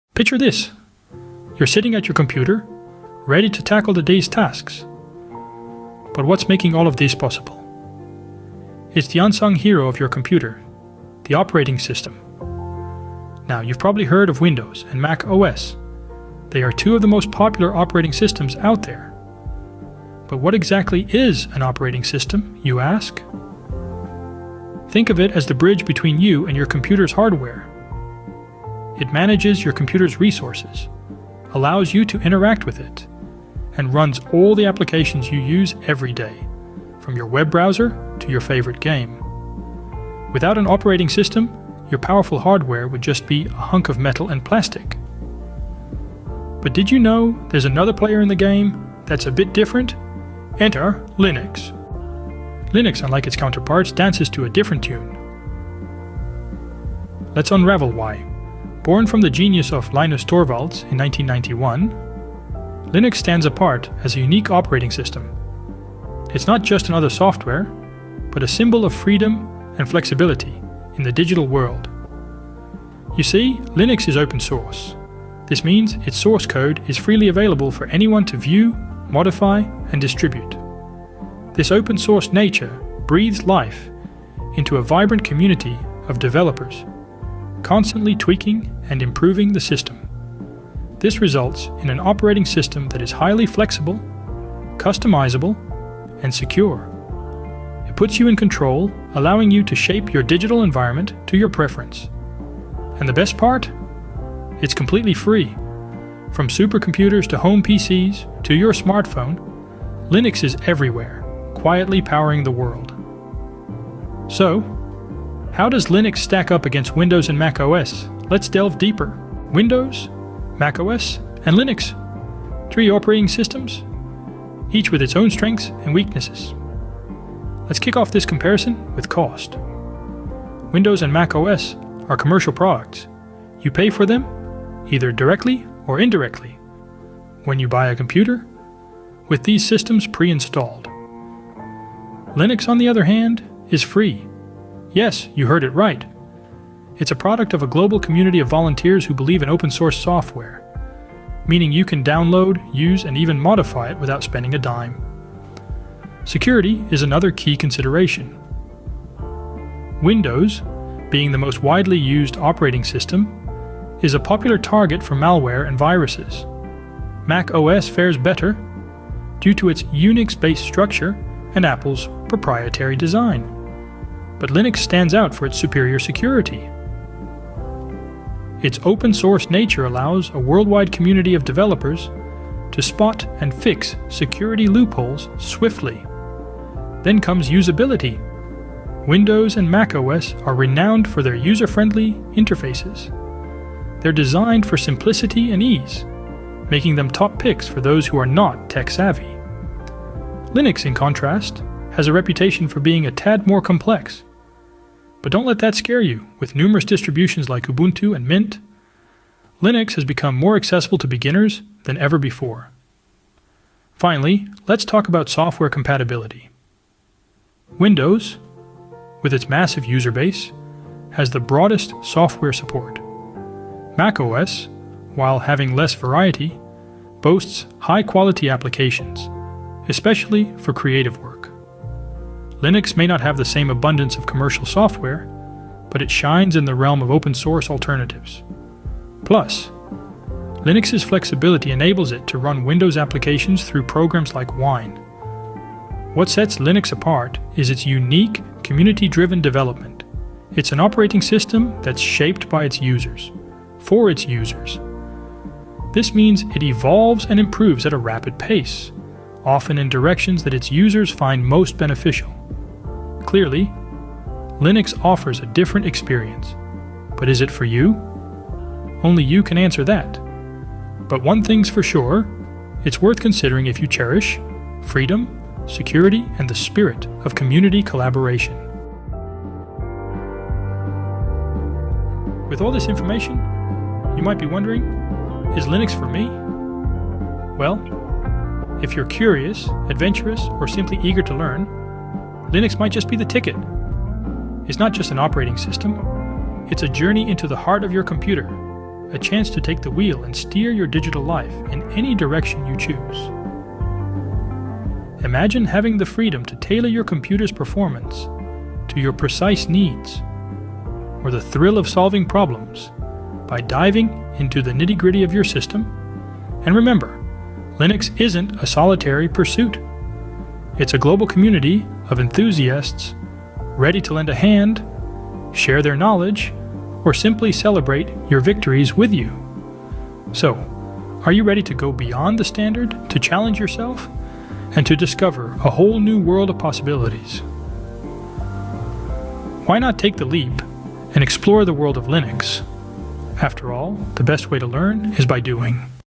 Linux: A Simple Guide for Beginners
In this video, we break down Linux for beginners in a clear and understandable way, comparing it with Windows and Mac OS. This is the perfect guide if you've ever been curious about Linux but felt intimidated. Join us as we explore the key differences between these operating systems in a simple, human language that anyone can understand. Whether you're a tech enthusiast or just starting out, this comparison will demystify Linux and help you make an informed choice for your computing needs.

OUTLINE:

00:00:00 Introduction to Operating Systems
00:00:51 Introduction to Linux
00:01:59 Comparing Linux with Windows and Mac OS
00:04:38 Encouragement to try Linux

Kindly share your thoughts, ideas, and questions in the comments section below, to help us improve this channel.